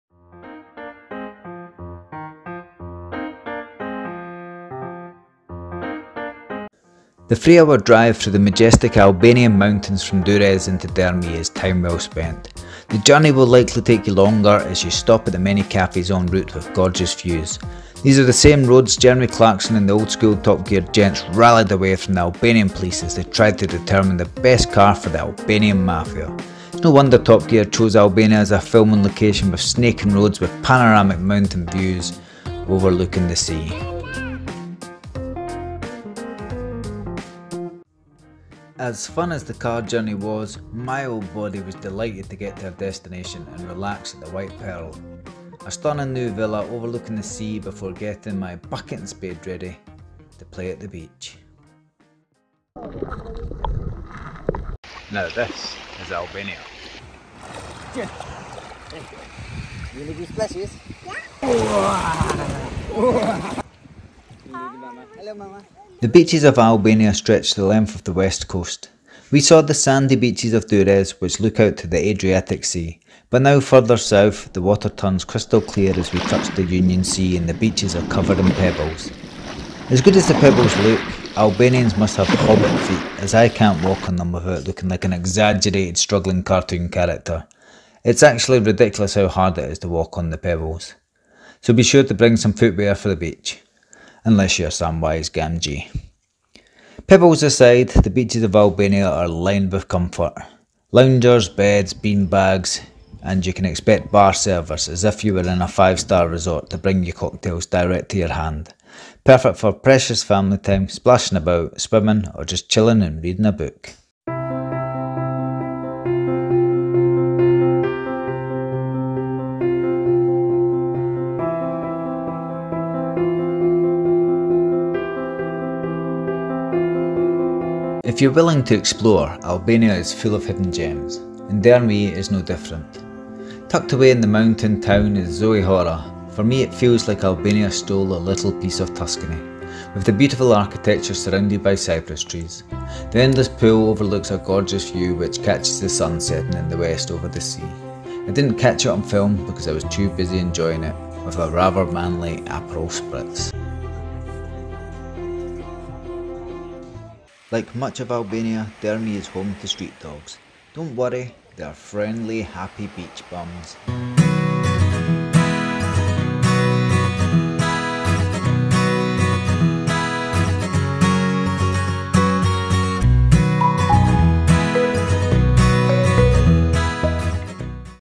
Exploring Dhërmi | Travel ALBANIA
Join us as we drive into the majestic mountains looking down over the pebble beaches of Dhermi.